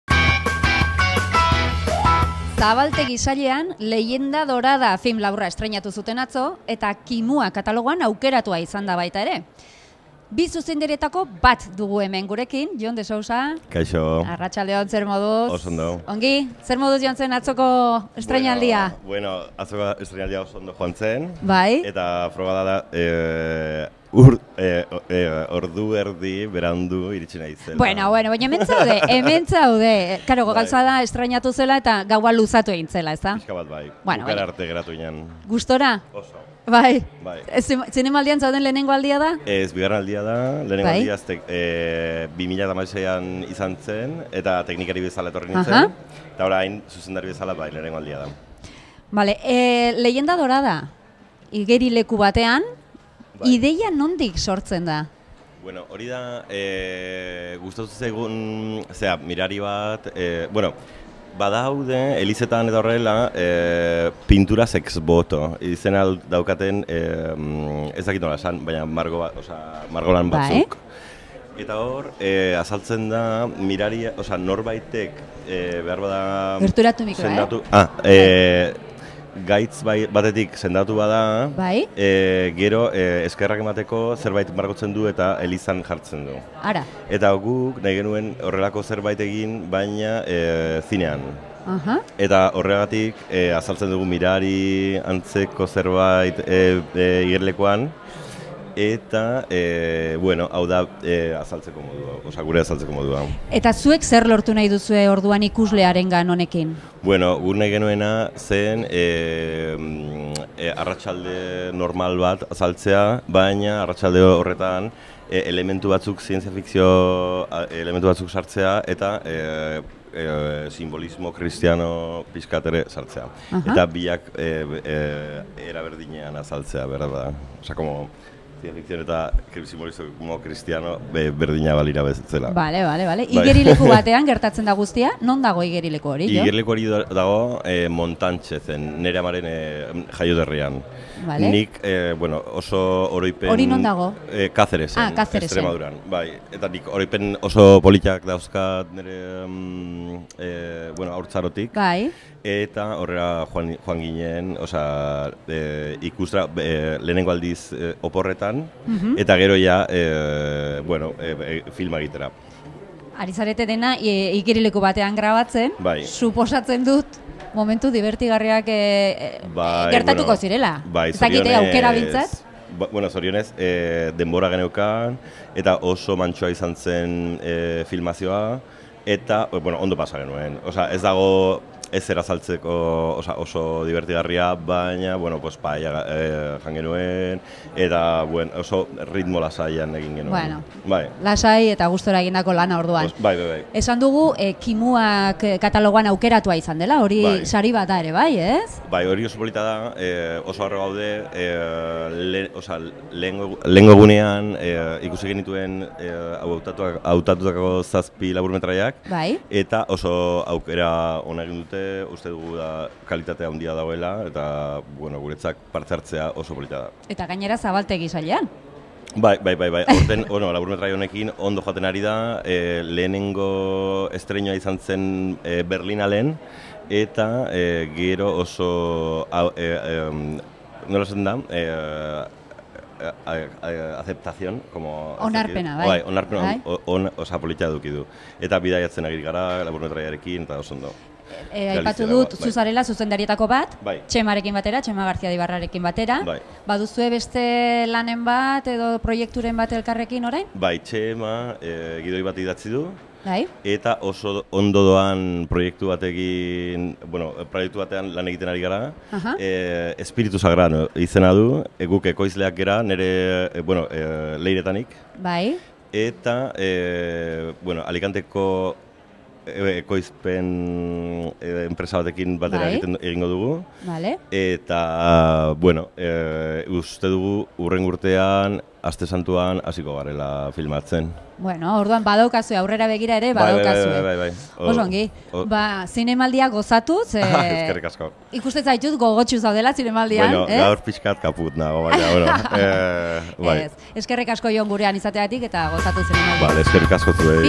Ion de Sosa Zurriolako Butakan
| Leyenda dorada film laburreko zuzendaria elkarrizketatu du Anabel Arraizak.